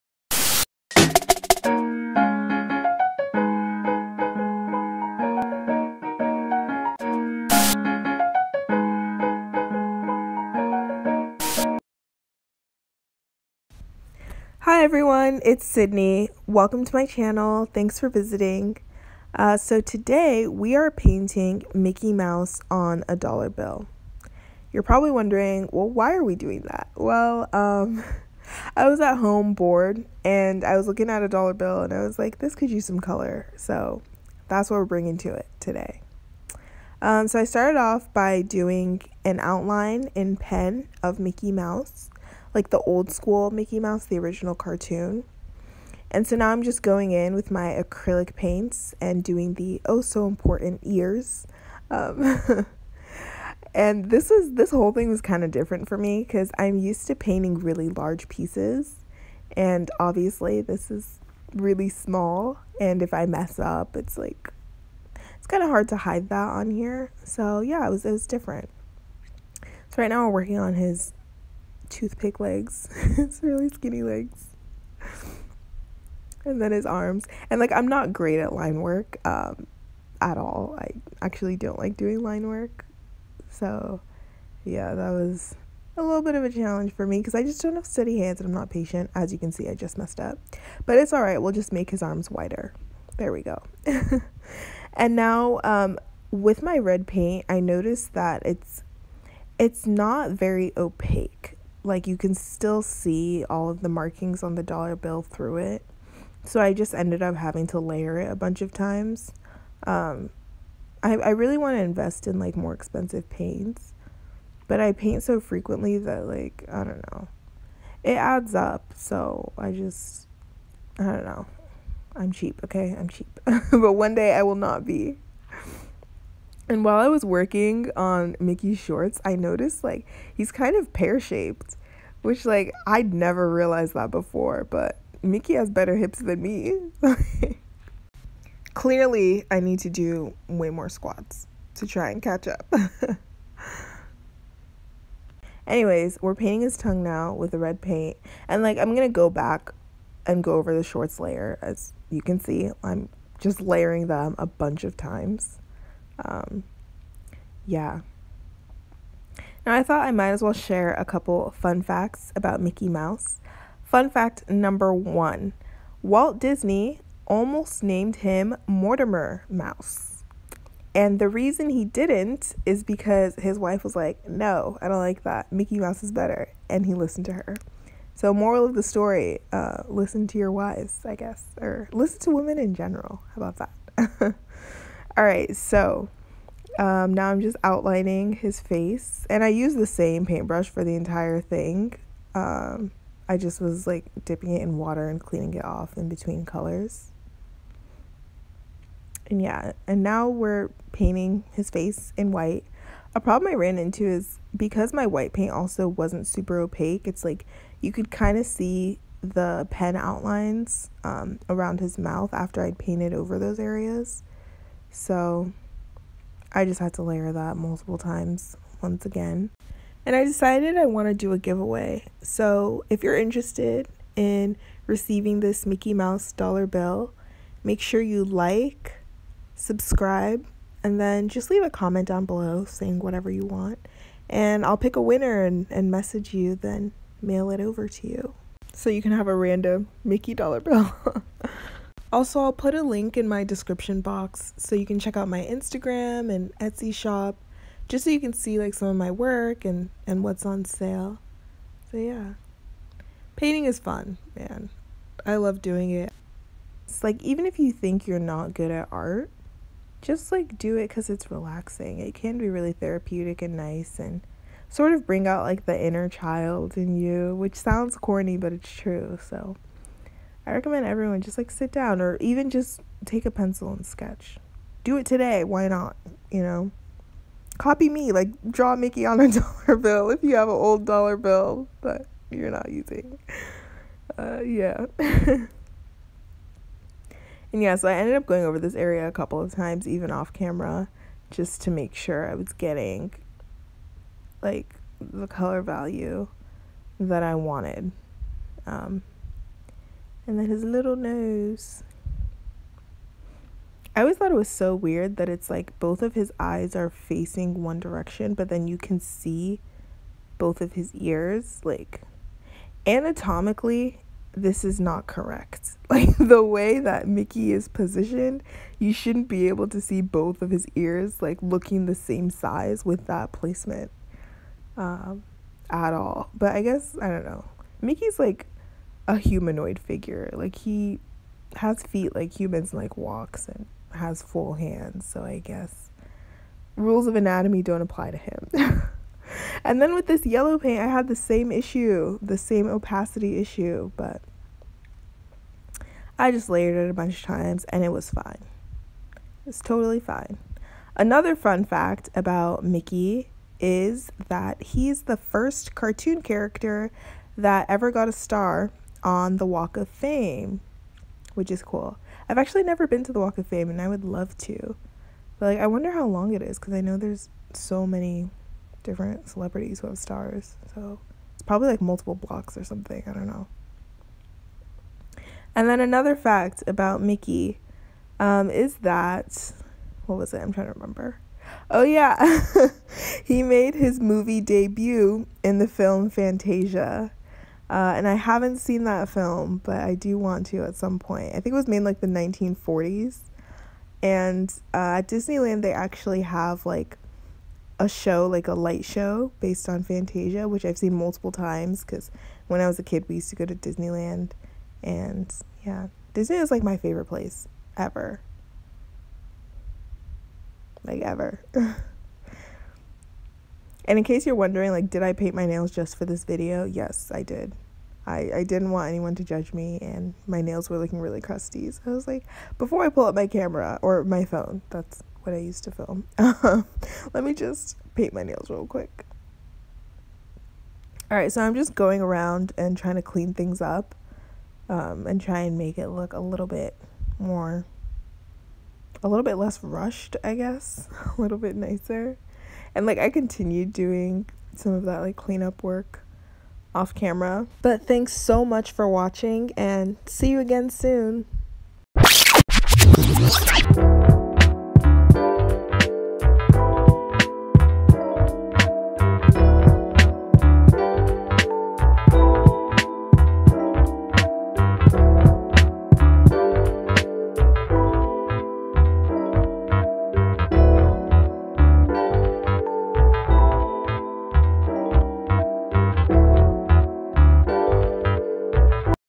PAINTING MICKEY MOUSE ON DOLLAR BILL (Art on REAL Money!)
Painting Mickey Mouse on a Dollar Bill. I was at home bored during quarantine, so I figured why not transform a dollar bill into some fun money art. Mickey mouse is a classic cartoon character, so I Immediately thought of him. In this video I show you the simple painting process, using acrylic paints. Hopefully watching this will make you want to pick up a pencil or some paints and make some art, just for fun as well. This was different for me because I typically do large realistic acrylic pieces.

I will choose a winner to receive the bill.**

Etsy Shop